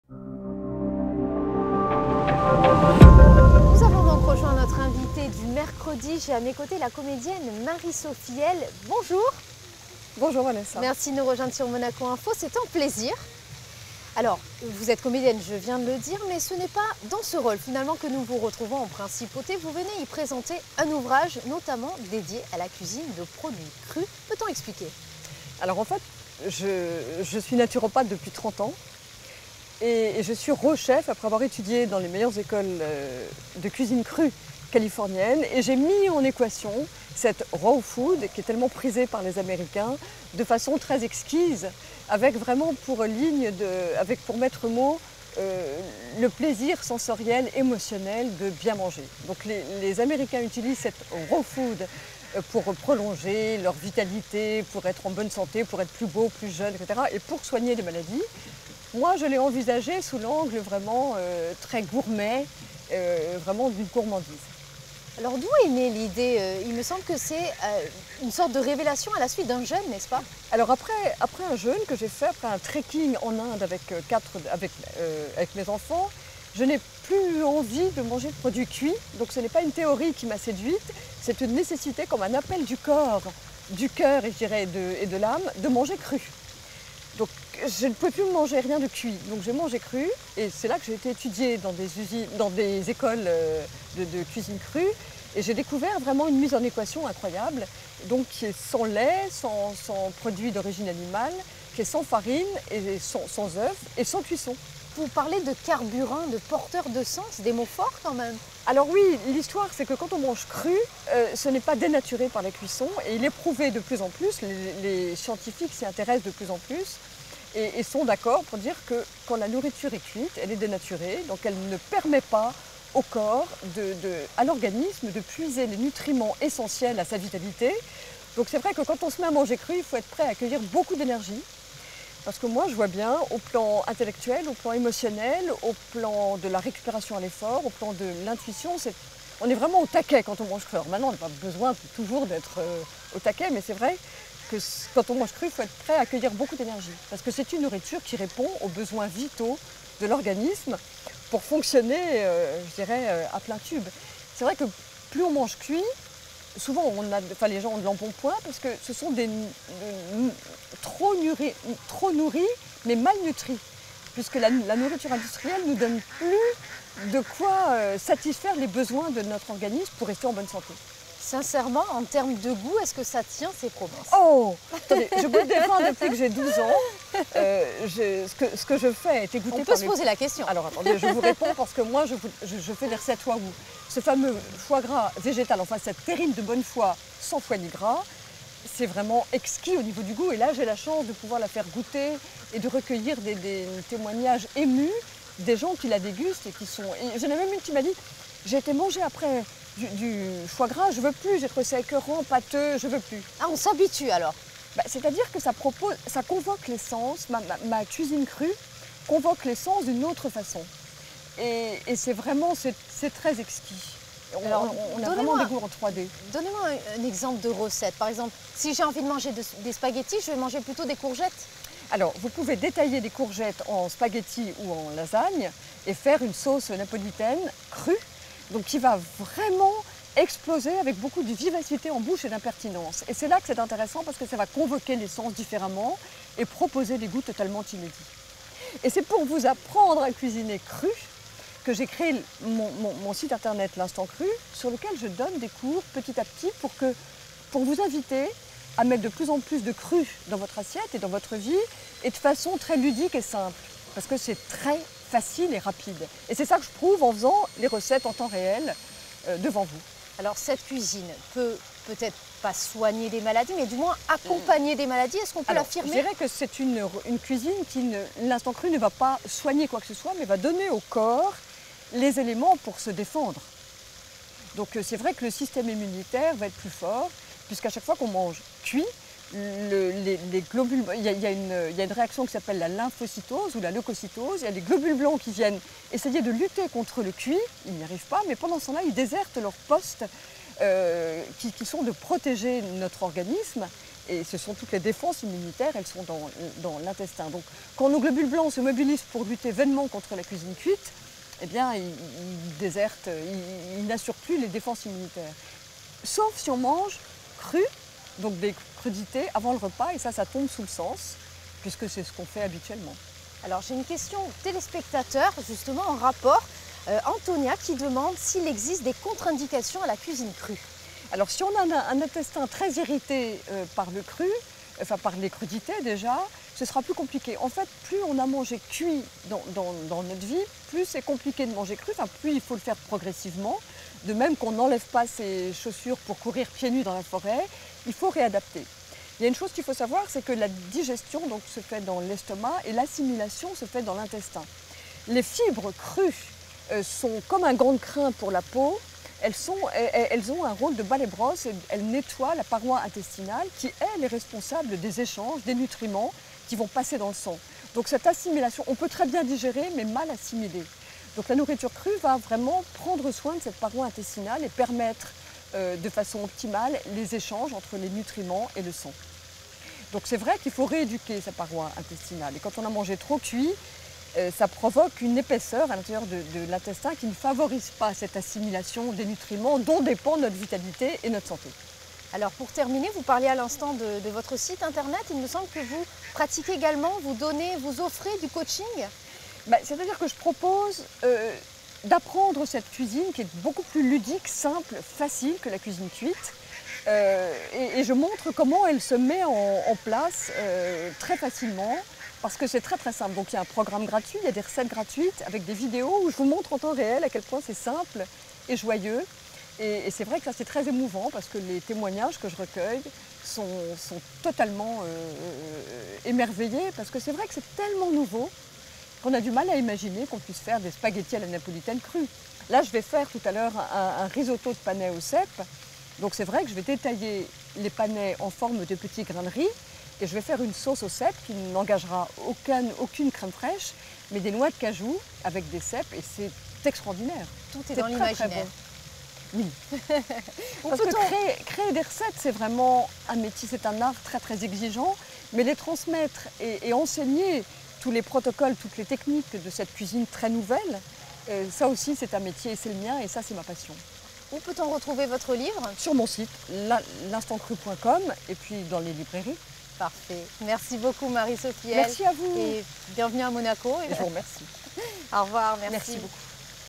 L’invitée du mercredi : Marie-Sophie L.
La comédienne Marie-Sophie L. est en Principauté pour présenter son ouvrage dédié à la cuisine de produits crus, intitulé « L’instant cru ». Également naturopathe depuis 30 ans, Marie-Sophie L. a développé un programme et des recettes à base de produits non cuits, sans farine et sans produits d’origine animale.